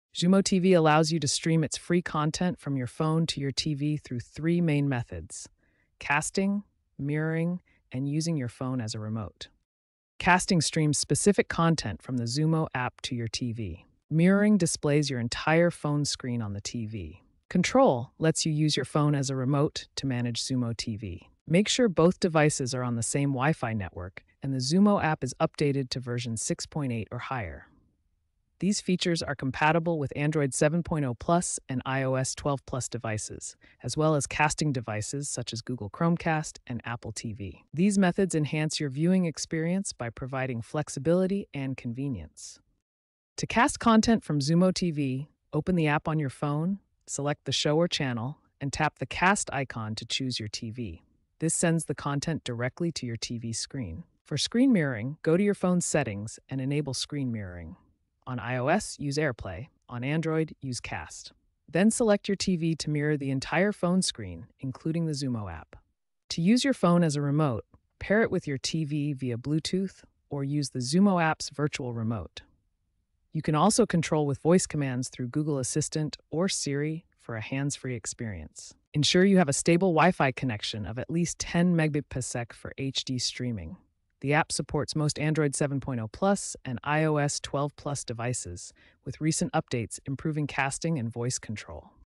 How to Connect Xumo TV to Phone – Cast, Mirror & Control
In this comprehensive tutorial, we will guide you through the process of connecting your Xumo TV to your smartphone. Whether you want to cast your favorite shows, mirror your phone's screen, or control your TV directly from your device, this video covers all the essential steps. We will explore various methods to ensure a seamless connection, including Wi-Fi setup and app usage. Perfect for both beginners and tech-savvy users, this guide will enhance your viewing experience.